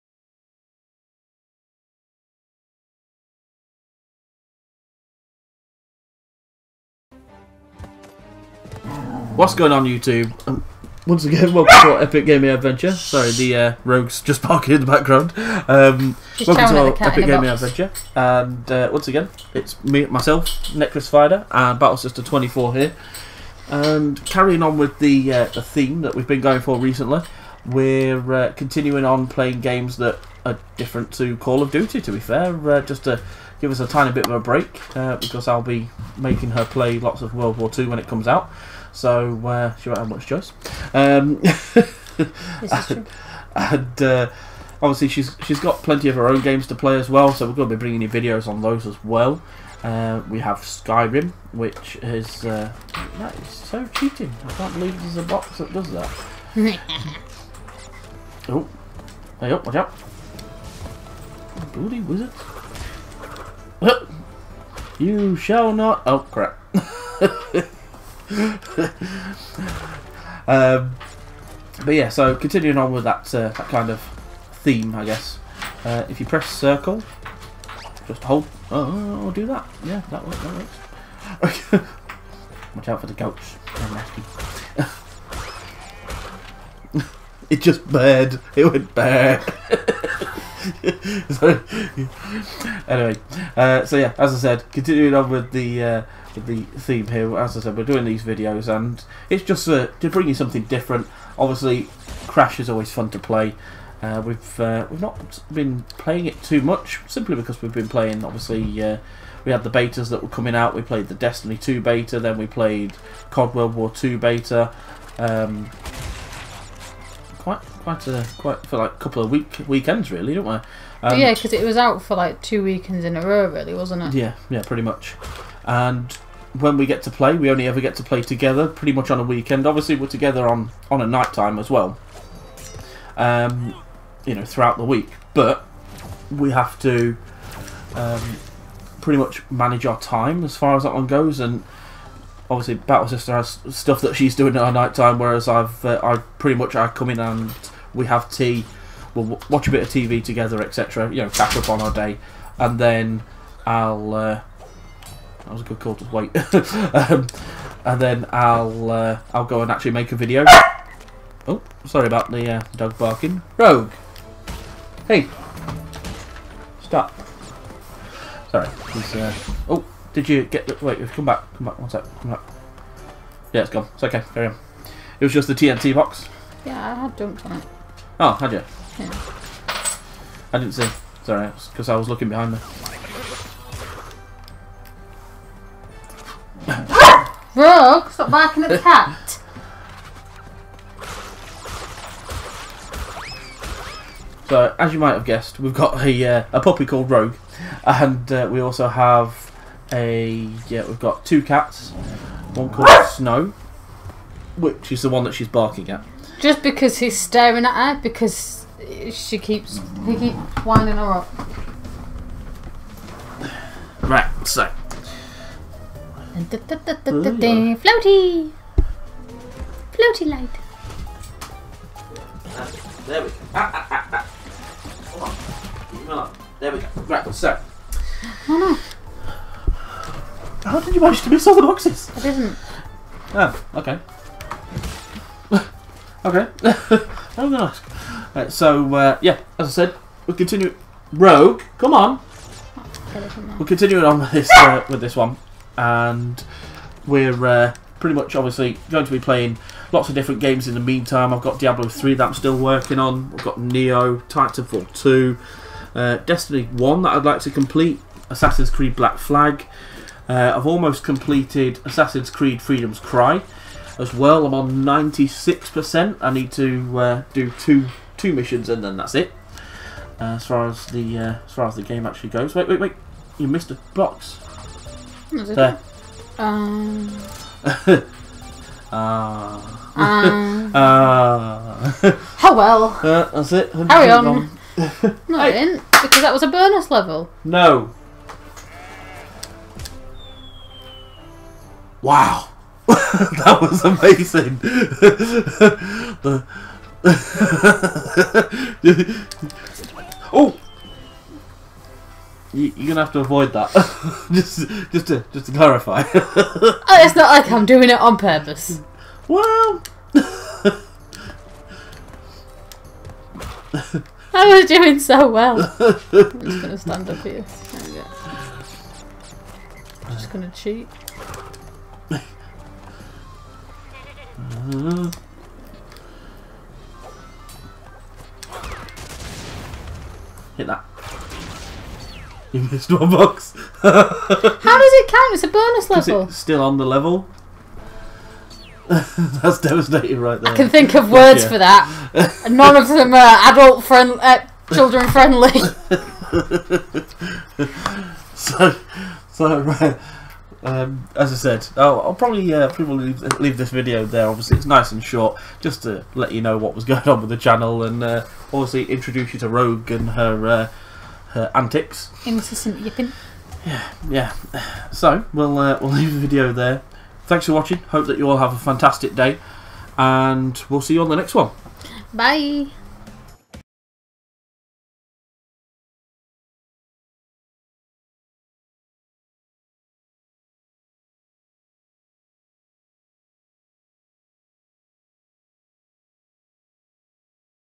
A crash course in meeting Rogue
Join Necris and Battlesister as they talk about the upcoming games on the channel and you get a very loud introduction to Rogue. (Puppy)

Sorry for the dog barking. She's young.